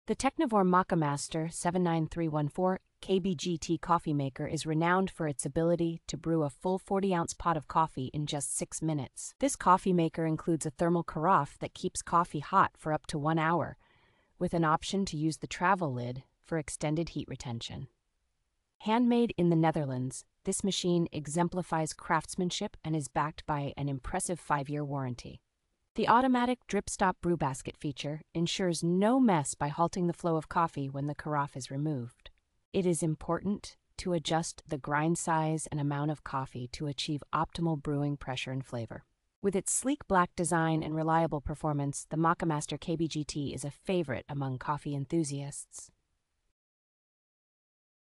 REVIEW (2024): Technivorm Moccamaster 79314 KBGT Coffee Maker. ESSENTIAL details.
The Technivorm Moccamaster 79314 KBGT Coffee Maker is renowned for its ability to brew a full forty ounce pot of coffee in just six minutes.

This coffee maker includes a thermal carafe that keeps coffee hot for up to one hour, with an option to use the travel lid for extended heat retention.

Handmade in the Netherlands, this machine exemplifies craftsmanship and is backed by an impressive five-year warranty.

The automatic drip-stop brew-basket feature ensures no mess by halting the flow of coffee when the carafe is removed.

It is important to adjust the grind size and amount of coffee to achieve optimal brewing pressure and flavor.

With its sleek black design and reliable performance, the Moccamaster KBGT is a favorite among coffee enthusiasts.